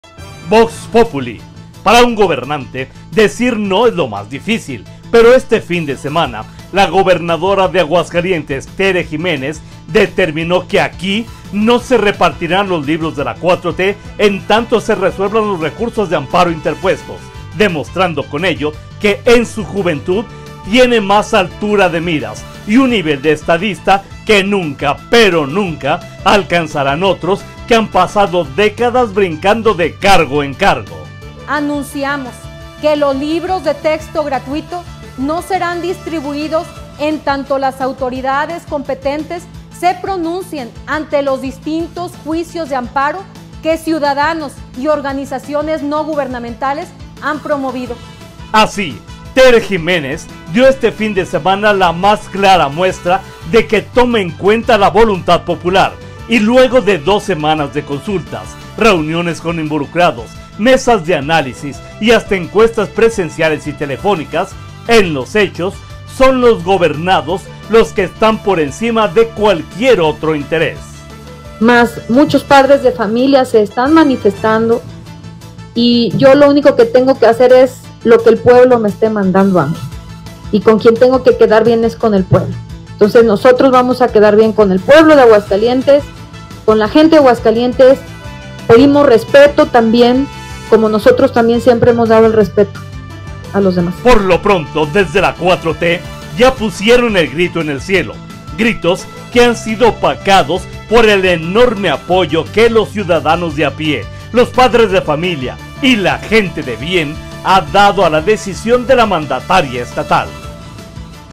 Crónica del Poder/Vox Populi
Vox Populi

Aguascalientes, Ags. 14 agosto 2023.- Para un gobernante, decir no es lo más difícil, pero este fin de semana la gobernadora de Aguascalientes, Tere Jiménez, determinó que aquí no se repartirán los libros de la 4T, demostrando con ello que en su juventud tiene más altura de miras y un nivel de estadista que nunca alcanzarán otros que han pasado décadas brincando de cargo en cargo.
Tere Jiménez Esquivel dio este fin de semanas la más clara muestra de tomar en cuenta la voluntad popular y, luego de dos semanas de consultas, reuniones con involucrados, mesas de análisis y hasta encuestas presenciales y telefónicas, en los hechos dejó claro que son sus gobernados los que están por encima de cualquier otro interés
Por lo pronto, desde la 4T ya pusieron el grito en el cielo, pero han sido opacados por el enorme apoyo que los ciudadanos de a pie, los padres de familia y la gente de bien ha dado a la decisión de la mandataria estatal.